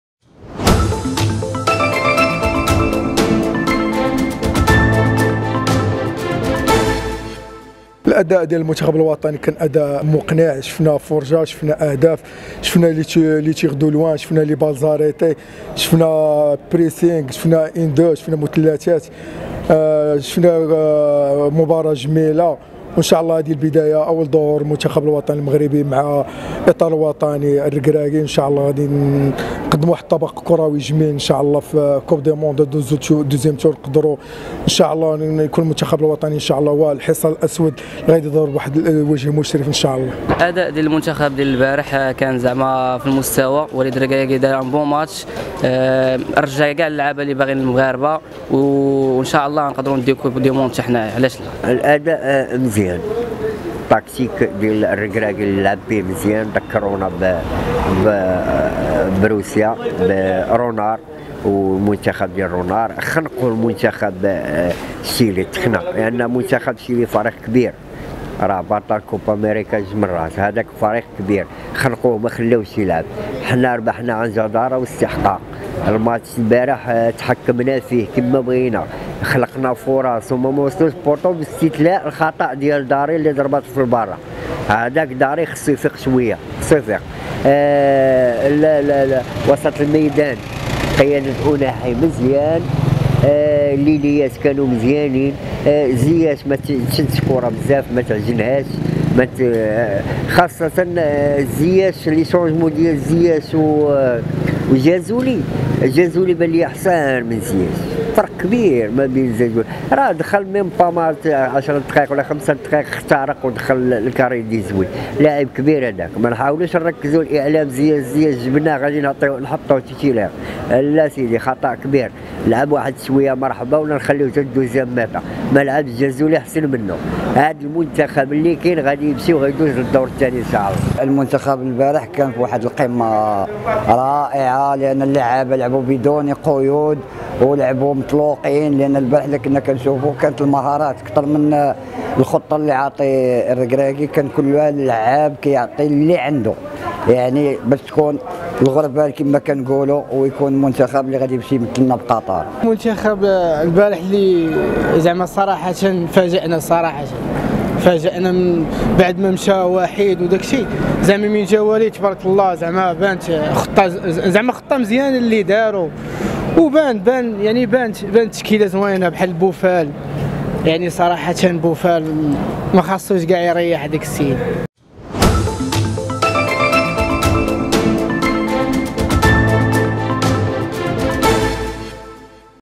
مواطنون: حنا فرحانين بالمدرب الركراكي وشفنا كلشي في الماتش
في هذا الفيديو عبر مجموعة من المواطنين عن سعادتهم بأداء المنتخب المغربي ضد الشيلي، مؤكدين أن وليد الركراكي نجح في إيجاد التوليفة المنافسة

# أخبار_عاجلة